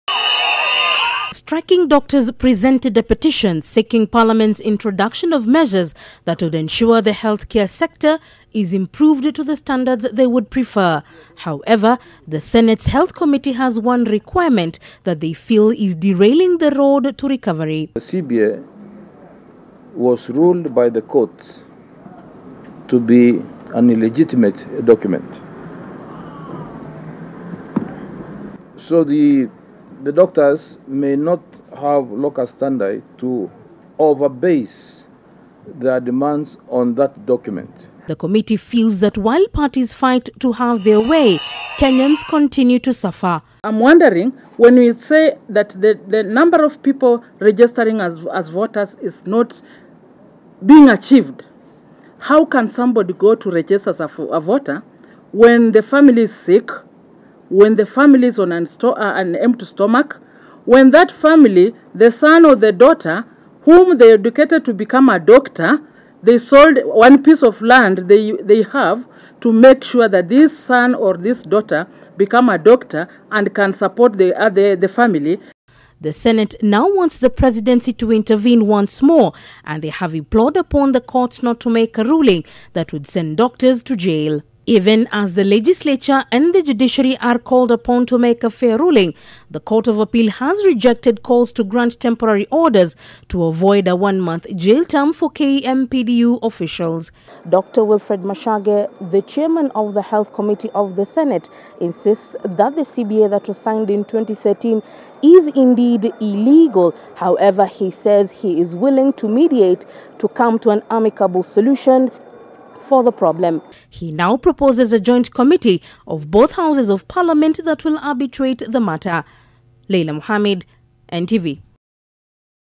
Senate offers helping hand in ending doctors' strike
"Stop insisting on an illegal "CBA " and come to the negotiating table with an open mind. " That was the Senate's Health Committee's plea to striking doctors, as the team volunteered to mediate talks between doctors and the government, for a solution. Committee Chair, Dr. Wilfred Machage, now says the CBA doctors want implemented, has been overtaken by events, and will derail efforts to unlock the deadlock, that has created a 59 day crisis.

The striking Doctors, who Dr Machage wants to come to the negotiating table, suffered a blow this morning, when the court of appeal refused to grant them temporary orders to block a hanging jail sentence for 7 KMPDU officials. Leila Mohammed has more.